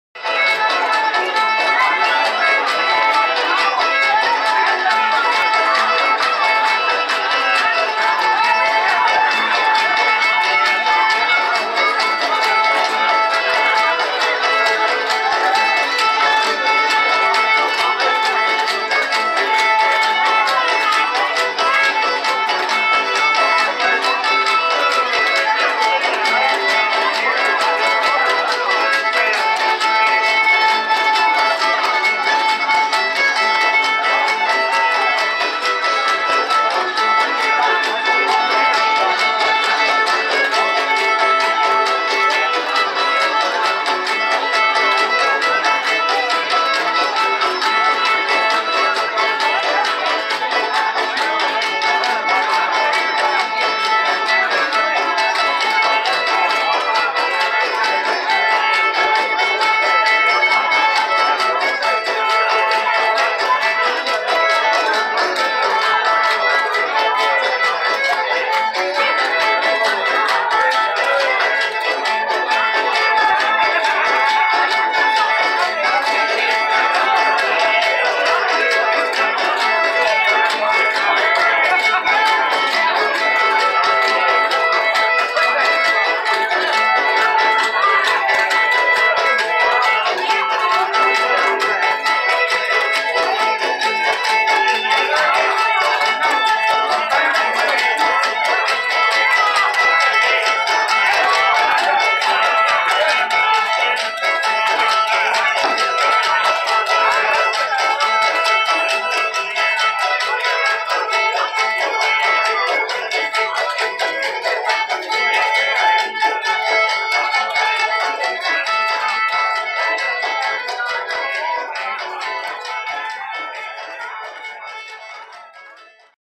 Shuffle at Alleghany Jubilee on a Sat. night
Music by: Mark Handy/Banjo, Roger Stamper/Fiddle, C.T. Janney/Washboard, Leon Frost/Mandolin, Stacy Boyd/Bass, Claude. /Guitar